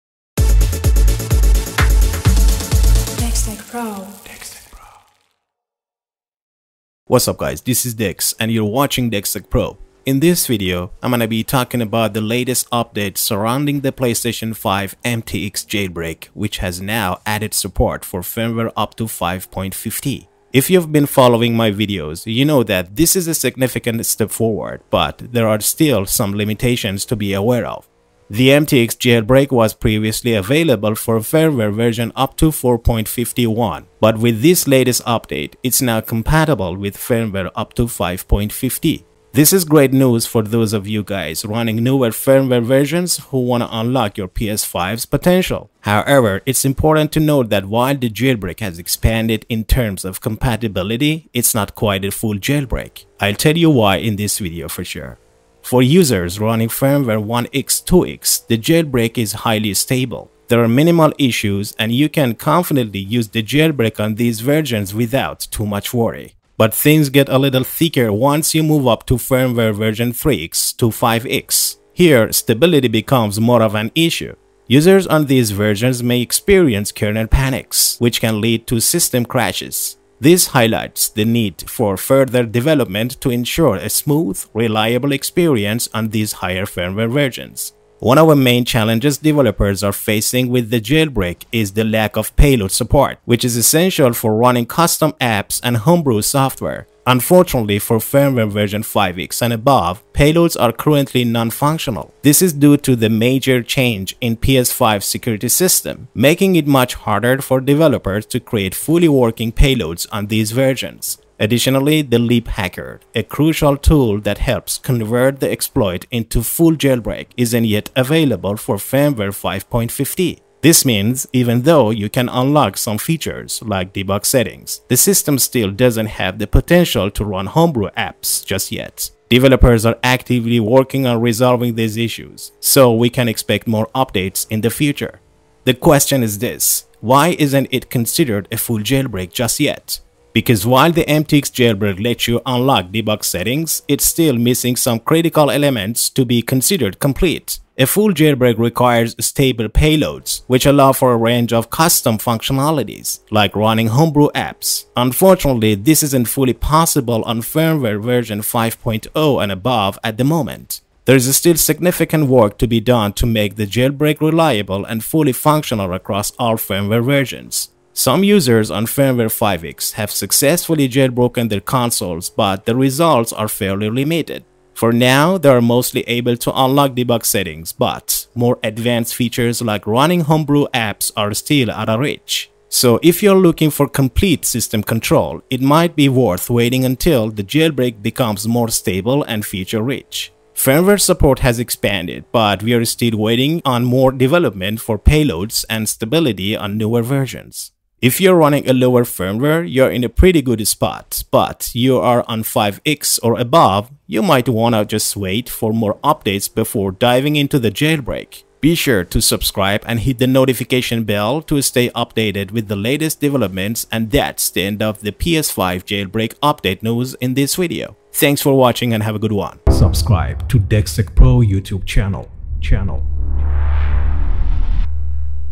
PS5 Jailbreak Ver 5.50 Update News
In this video, we explore the latest updates on the PS5 MTX jailbreak, which now supports firmware versions up to 5.50.

I'll break down what this means for users, discuss the stability across different firmware versions, and dive into the ongoing challenges with payload support.

While the jailbreak allows users to unlock debug settings, it’s not yet a full jailbreak, especially for those on newer firmware.